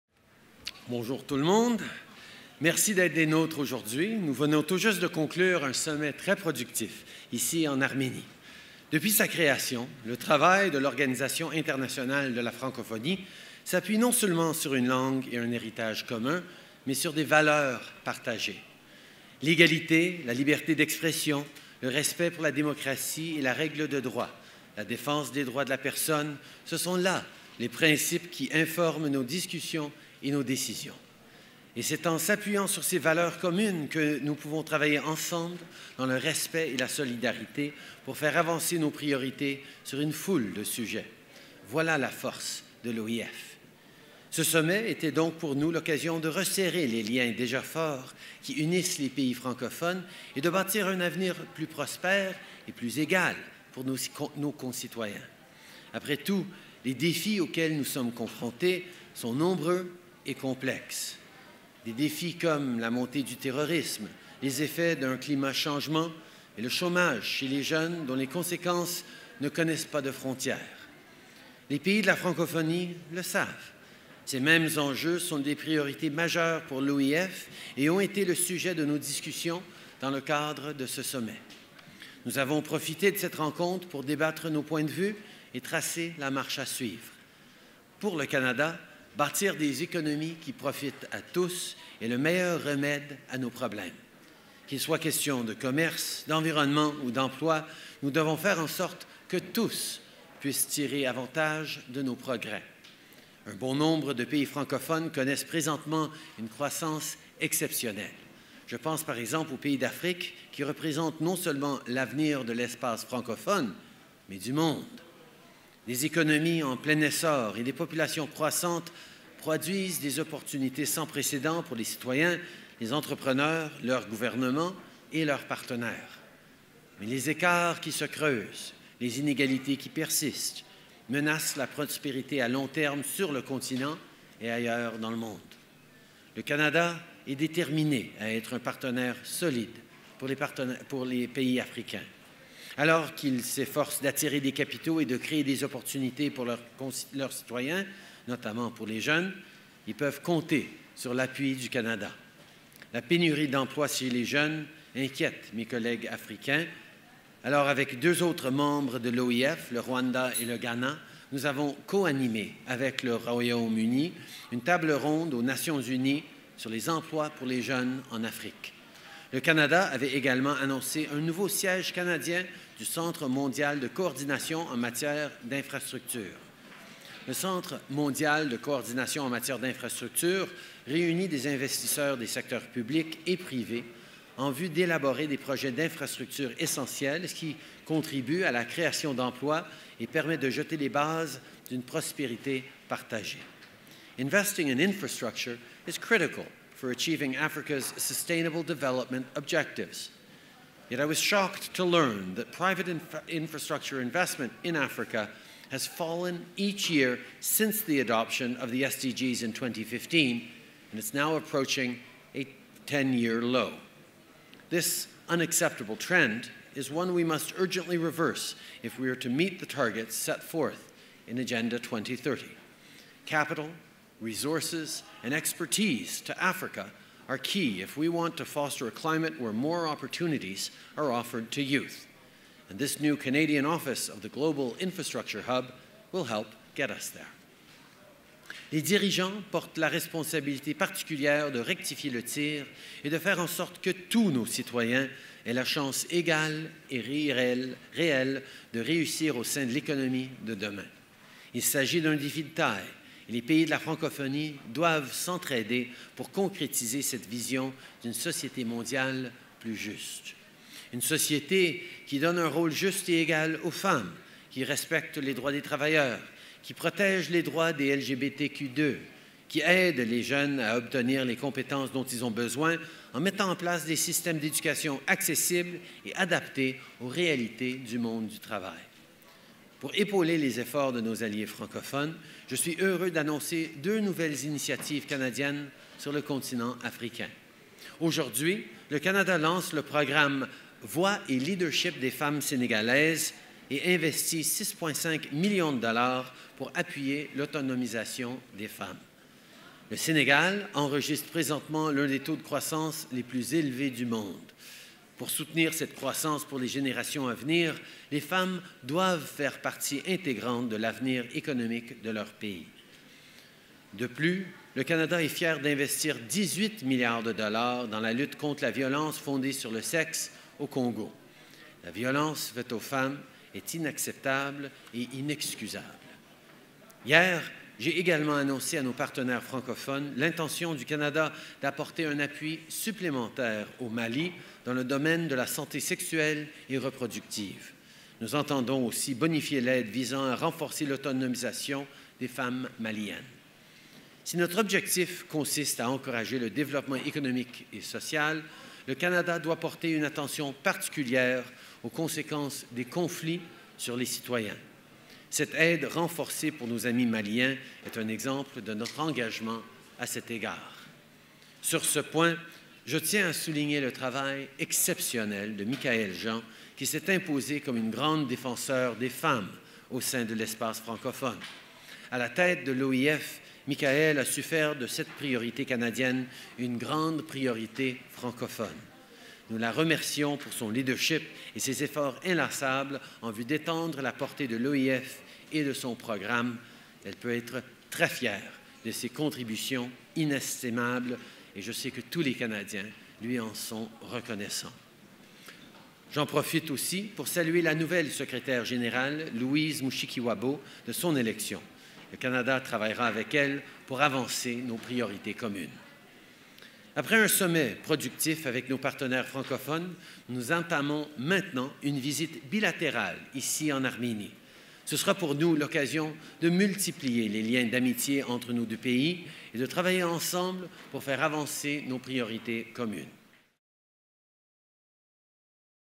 PM Trudeau delivers remarks following the conclusion of the Francophonie Summit in Yerevan, Armenia
Prime Minister Justin Trudeau delivers remarks to the media following the conclusion of the 17th Francophonie Summit in Yerevan, Armenia.